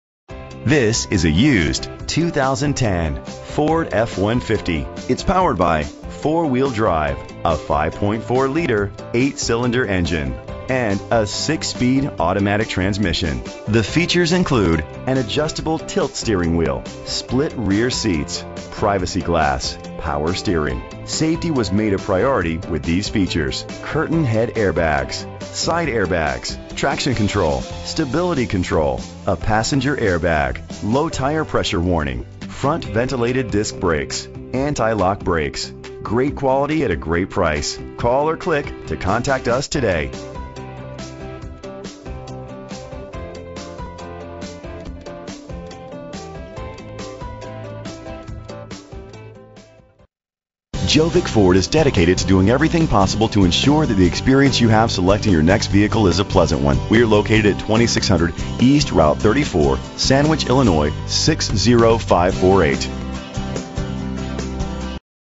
2010 Ford F-150 - Sandwich IL
Phone:
Year: 2010
Make: Ford
Model: F-150
Trim: XLT
Engine: 5.4 liter 8 cylinder 24 valve
Transmission: 6-Speed Automatic
Color: Blue
Mileage: 93885
Address:
2600 Route 34